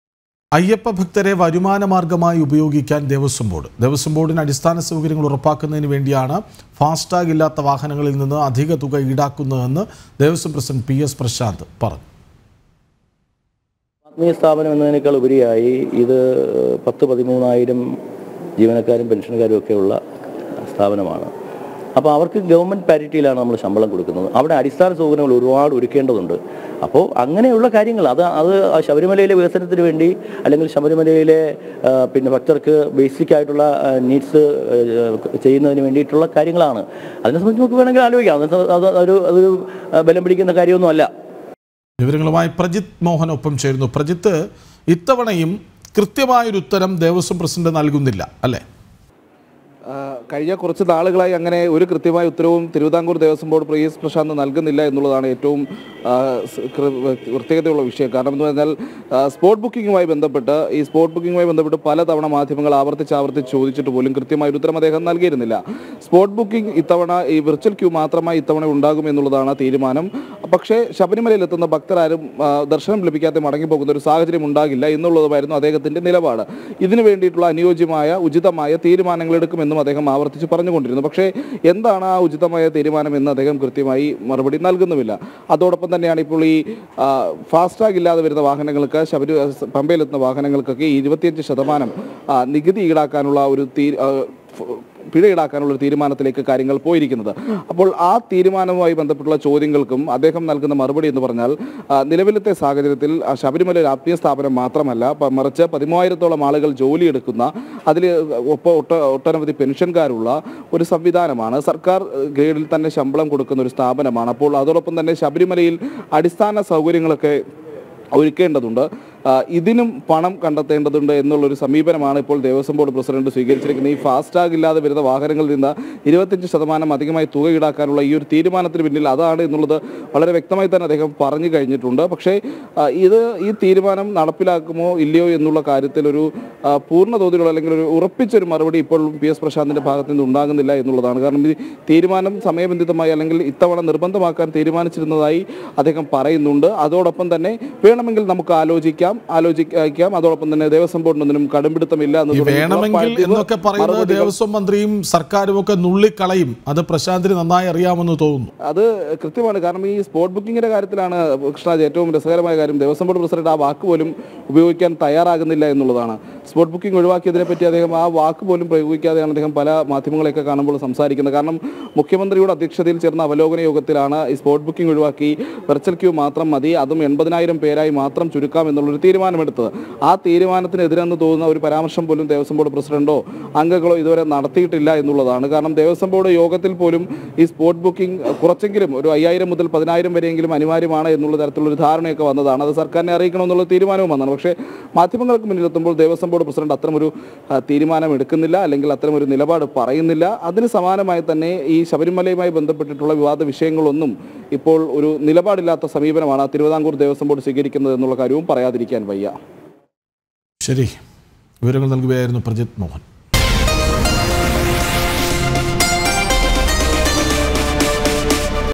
അയ്യപ്പ ഭക്തർക്ക് അധിക പിഴ; അടിസ്ഥാന സൗകര്യങ്ങൾ ഉറപ്പാക്കാനെന്ന വാദവുമായി ദേവസ്വം ബോർഡ്
അയ്യപ്പ ഭക്തരെ വരുമാനമാർഗമായി ഉപയോഗിക്കാന്‍ ദേവസ്വം ബോർഡ്. ദേവസ്വം ബോർഡിന് അടിസ്ഥാന സൗകര്യങ്ങൾ ഉറപ്പാക്കുന്നതിന് വേണ്ടിയാണ് ഫാസ്‍ടാഗ് ഇല്ലാത്ത വാഹനങ്ങളിൽ നിന്ന് അധിക തുക ഈടാക്കുന്നതെന്ന് ദേവസ്വം ബോർഡ് പ്രസിഡന്‍റ്  പി എസ് പ്രശാന്ത്  പറഞ്ഞു.